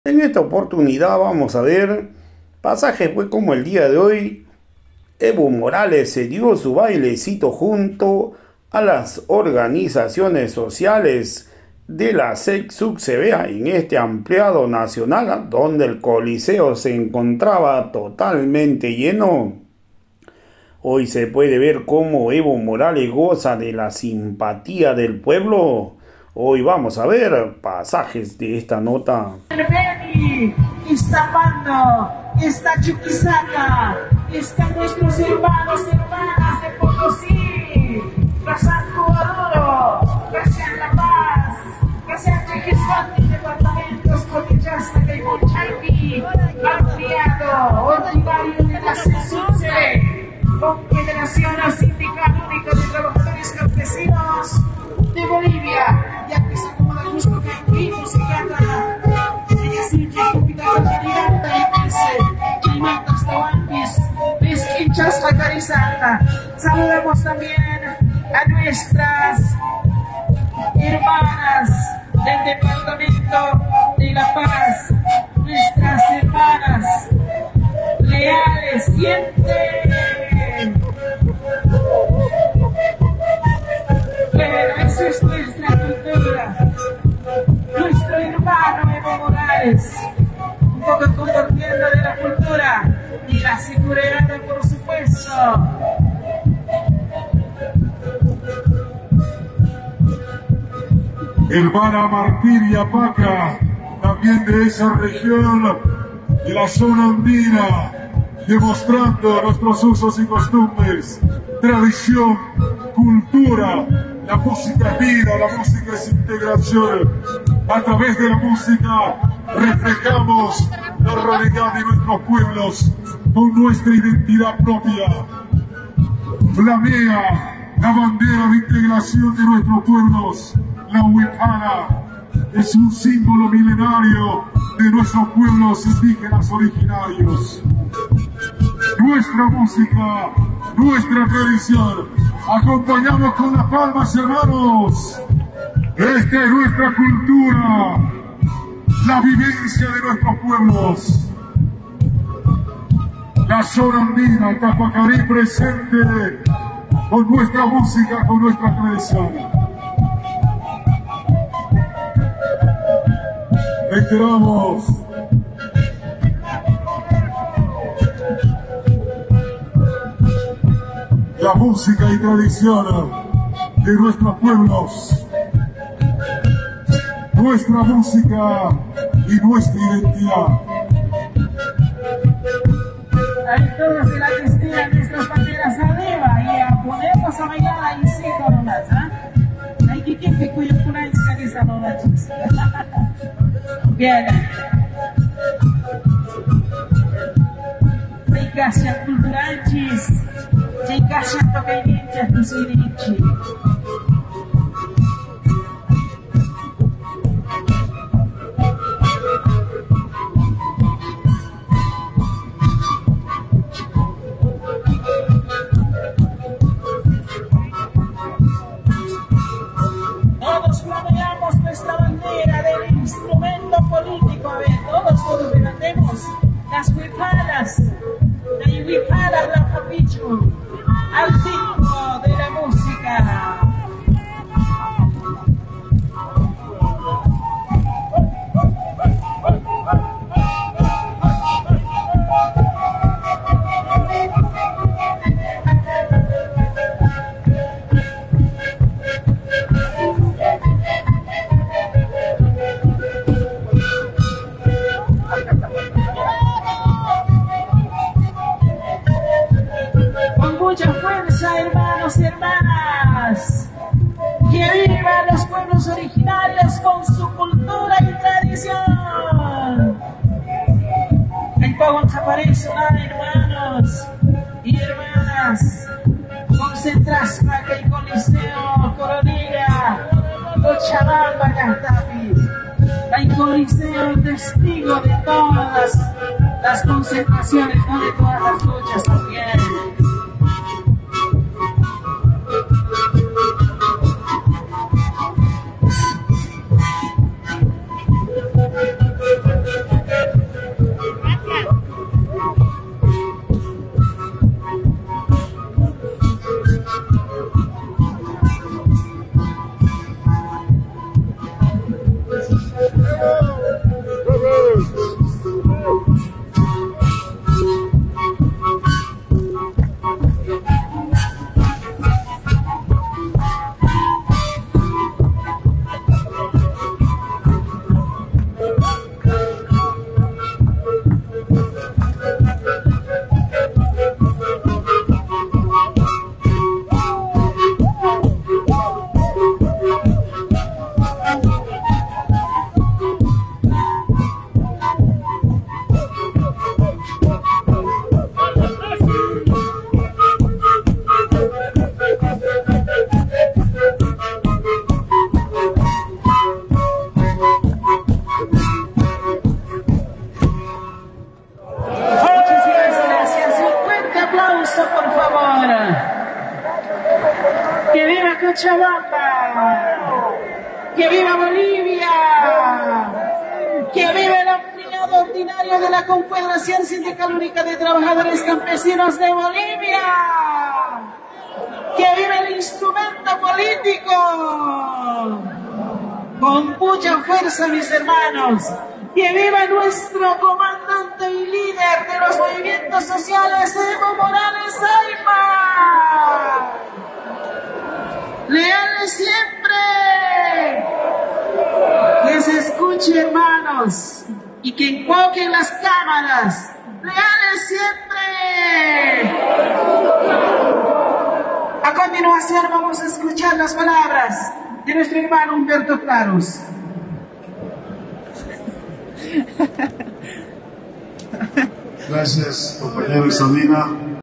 EVO MORALES SE DIO SU BAILE JUNTO A LAS ORGANIZACIONES EN AMOPLIADO CSUTCB..
CON MUCICA AUTONTONA SE DIO INICIO AMPLIADO NACIONAL CSUTCB-BOLIVIA..
Bolivia Ampliadonacional-csutcb#Evomorales#Evomoralesbailando.